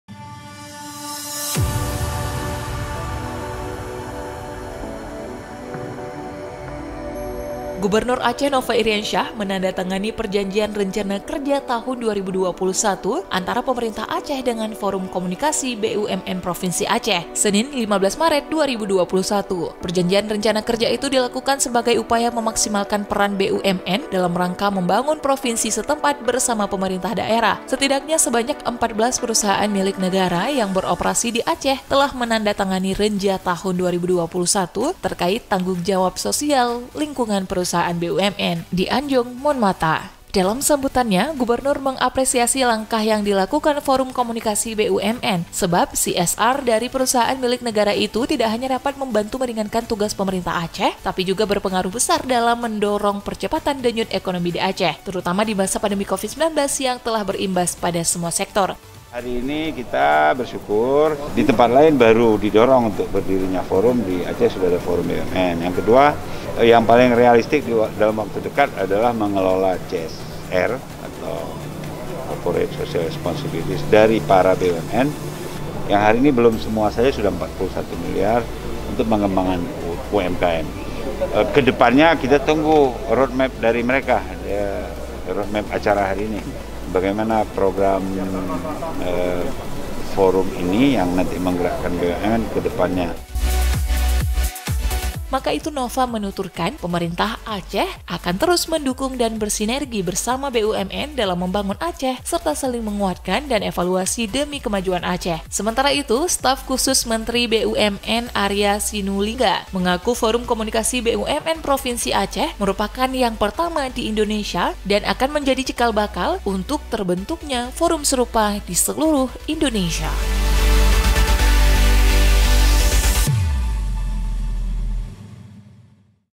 Nova Teken Renja TJSLP Tahun 2021 dengan FK BUMN
BANDA ACEH - Gubernur Aceh Nova Iriansyah, menandatanganil perjanjian Rencana Kerja tahun 2021 antara Pemerintah Aceh dengan Forum Komunikasi BUMN Provinsi Aceh, Senin (15/03/2021).

Perjanjian Rencana Kerja itu dilakukan sebagai upaya memaksimalkan peran BUMN dalam rangka membangun provinsi setempat bersama pemerintah daerah.

Setidaknya, sebanyak 14 perusahaan milik negara yang beroperasi di Aceh telah menandatangani Renja tahun 2021 terkait tanggung jawab sosial lingkungan perusahaan BUMN, Senin (15/3/2021), di Anjong Mon Mata.

Dalam sambutanya, Gubernur mengapresiasi langkah yang dilakukan Forum Komunikasi BUMN. Sebab CSR dari perusahaan milik negara itu, tidak hanya dapat membantu meringankan tugas Pemerintah Aceh, tapi juga berpengaruh besar dalam mendorong percepatan denyut ekonomi di Aceh, terutama di masa pandemi Covid-19, yang telah berimbas pada semua sektor.